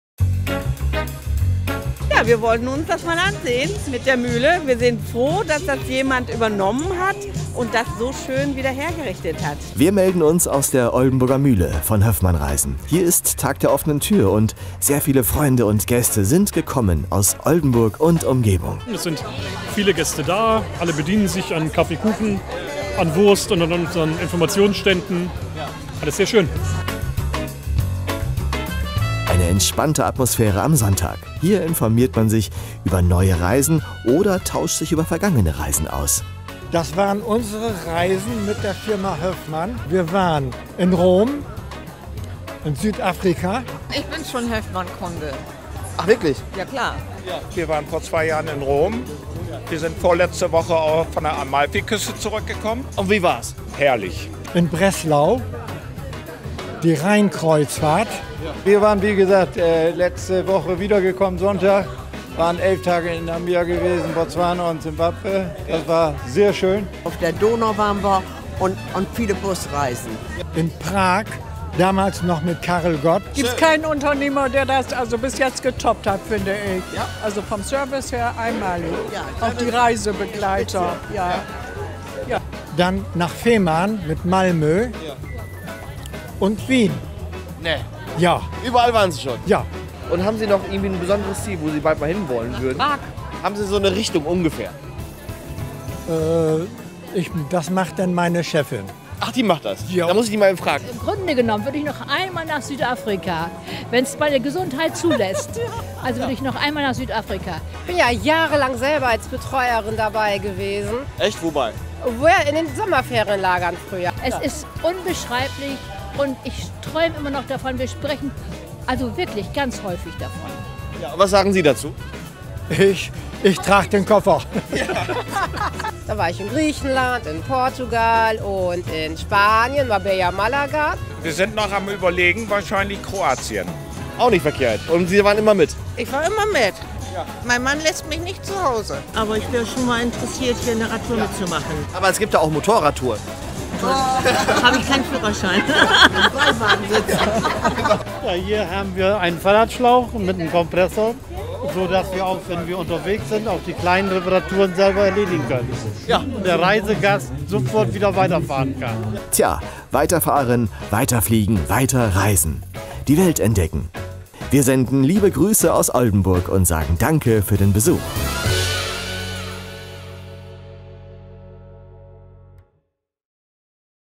Tag der offenen Tür in der Oldenburger Mühle von Höffmann Reisen
Ein großartiger Tag an unserem Standort in Oldenburg. Vielen Dank für den Besuch!